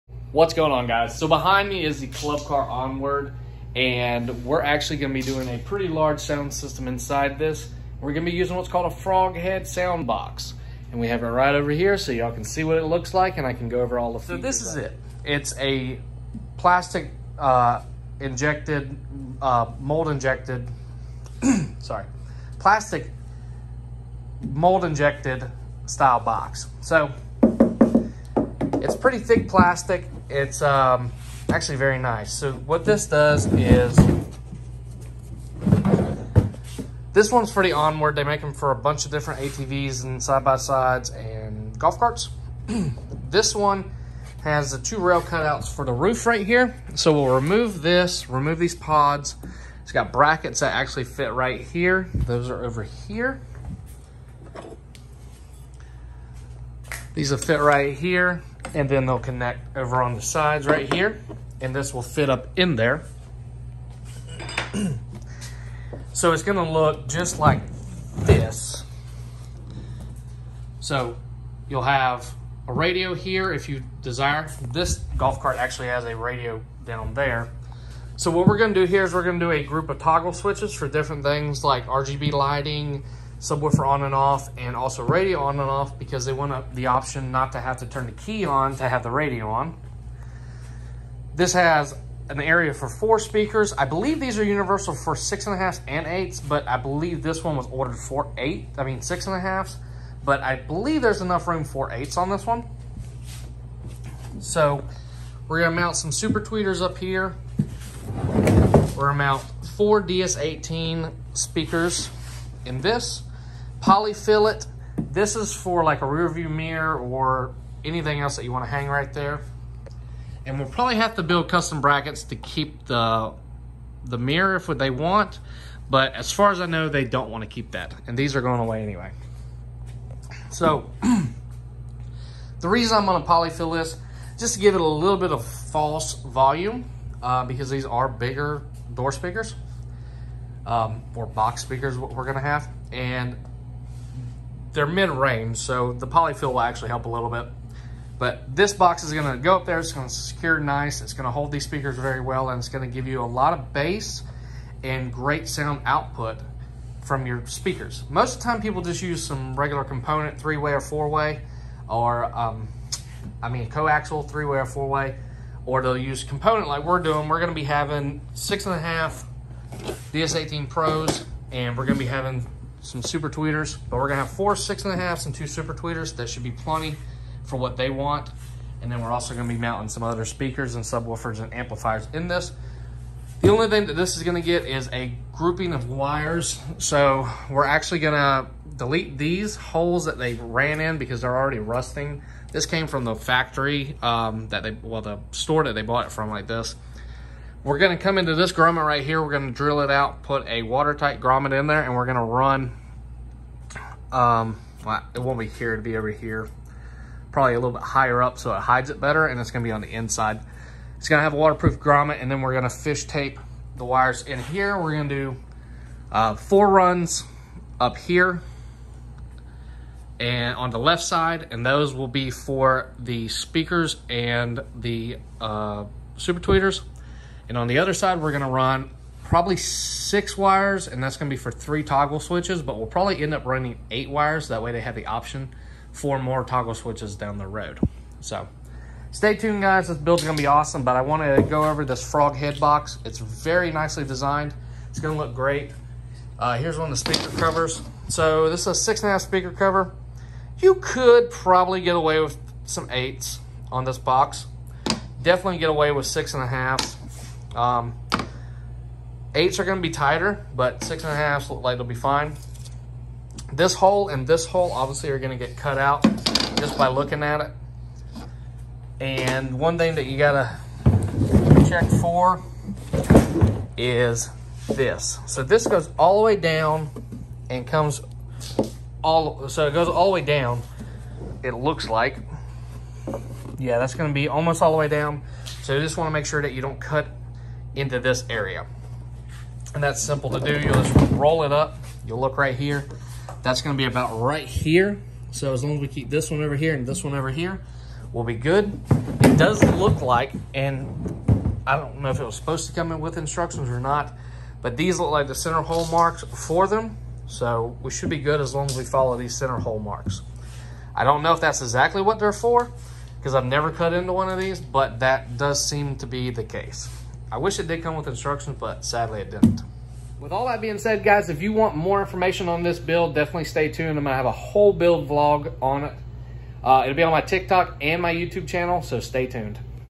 Club Car Onward FrogHead industries Blank Box Overview before install
In this video I wanted to show the frog head industries speaker box for the club car onward series golf cart. This is a none loaded enclosure so we will be adding our own speakers and radio to this to make it really get loud.